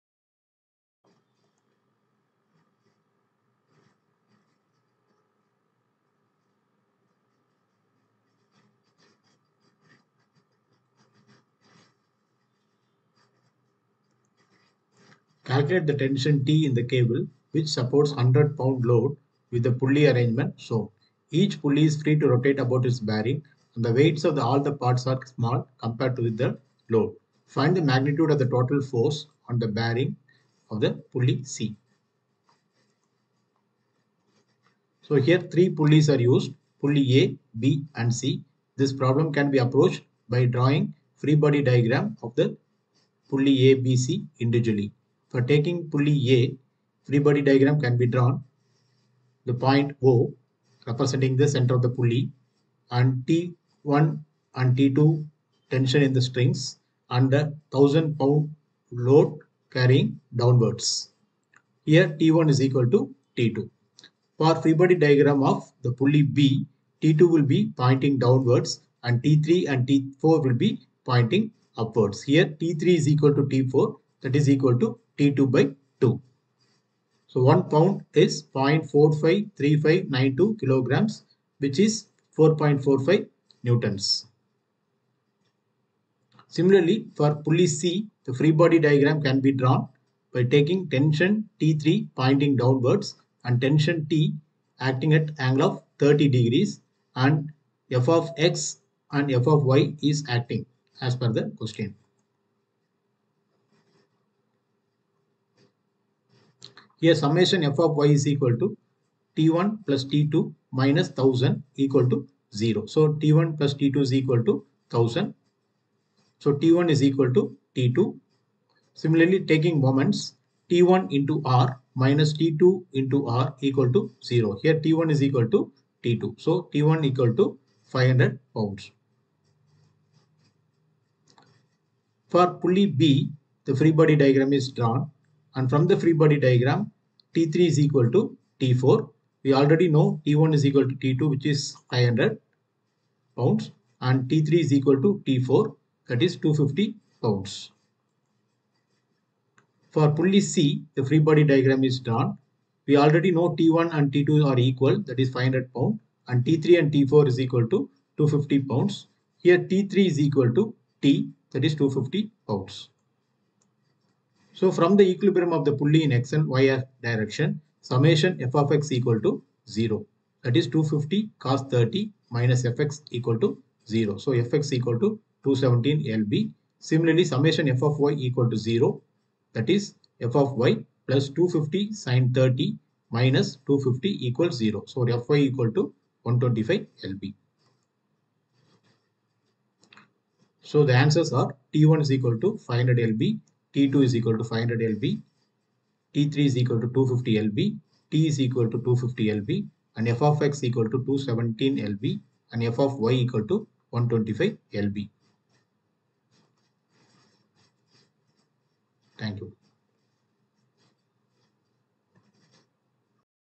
Equilibrium of Forces Pulley Problem
Calculate the tension T in the cable that supports the 1000-lb load with the pulley arrangement shown. Each pulley is free to rotate about its bearing, and the weights of all parts are small compared with the load. Find the magnitude of the total force on the bearing of pulley C.